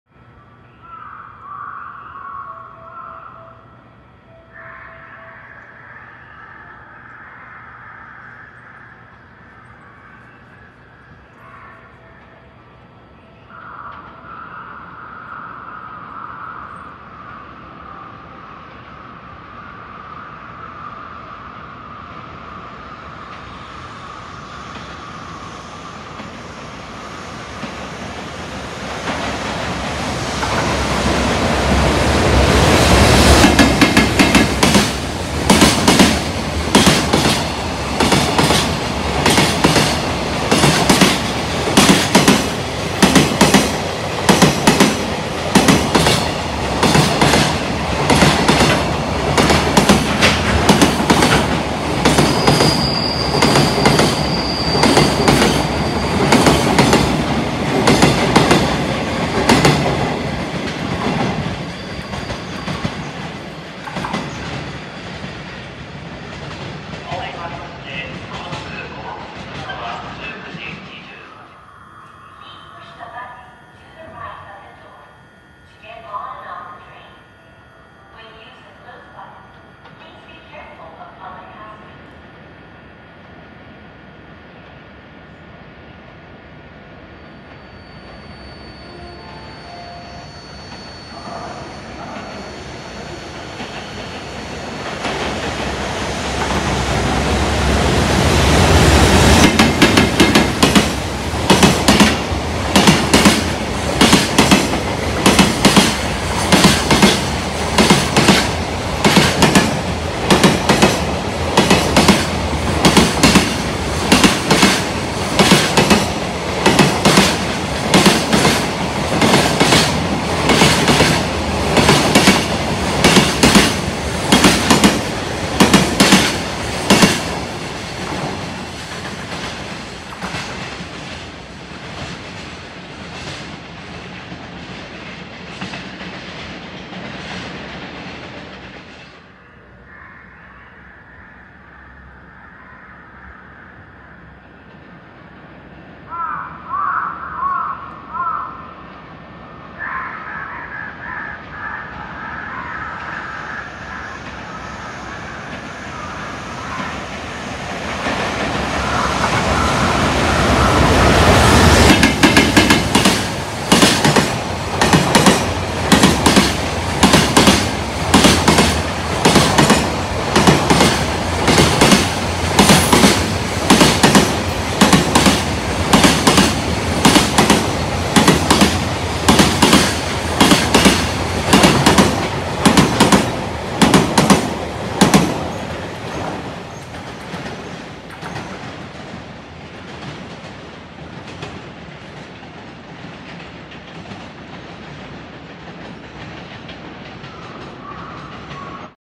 赤・銀・青のEF510 貨物列車 長岡駅通過 豪快ジョイント音パネェ〜ぜ！
2024/5/25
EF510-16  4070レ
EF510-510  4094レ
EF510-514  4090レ